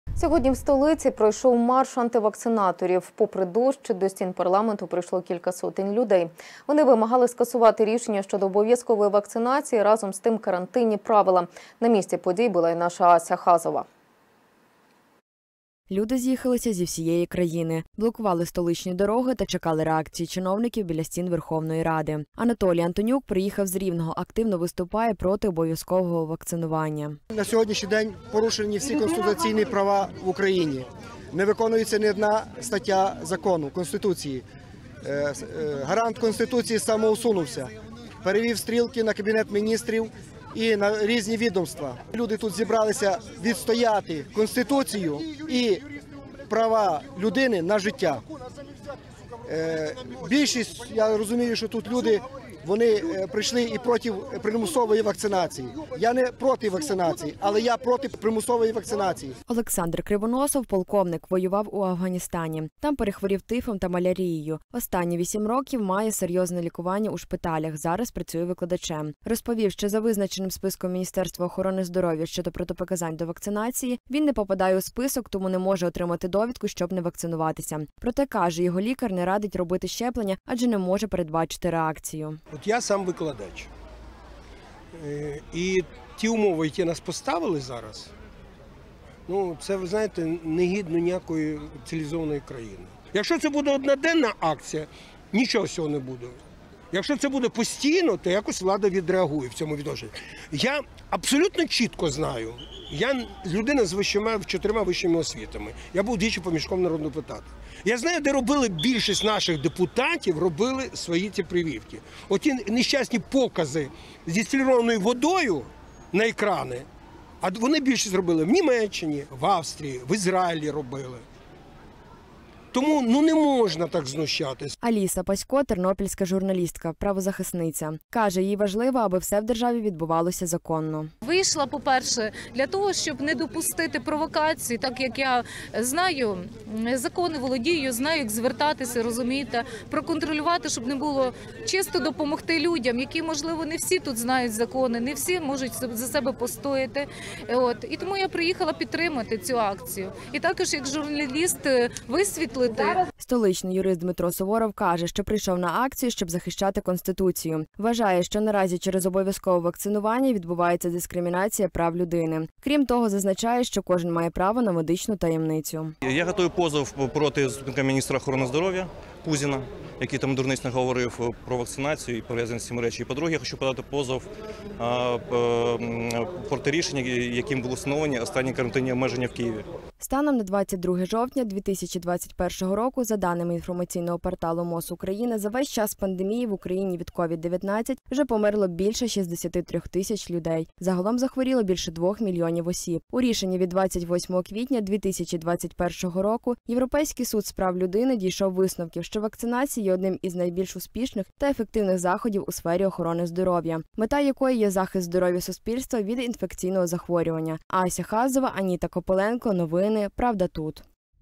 У Києві біля Верховної Ради люди зі всієї України вимагають відмінитибажану вакцинацію
З 8 години ранку біля Верховної Ради України відбувається мирна акція протесту. Люди зі всієї України з’їхалися вимагати від Кабміну відмінити, як вони називають, примусову вакцинацію. Під час акції пікетувальники вирішили створити “Народну самооборону”, орган, який захищатиме Закон і Конституцію. Серед учасників є юристи, громадські діячі, викладачі. Учасники протесту перекривали дороги поблизу Верховної Ради.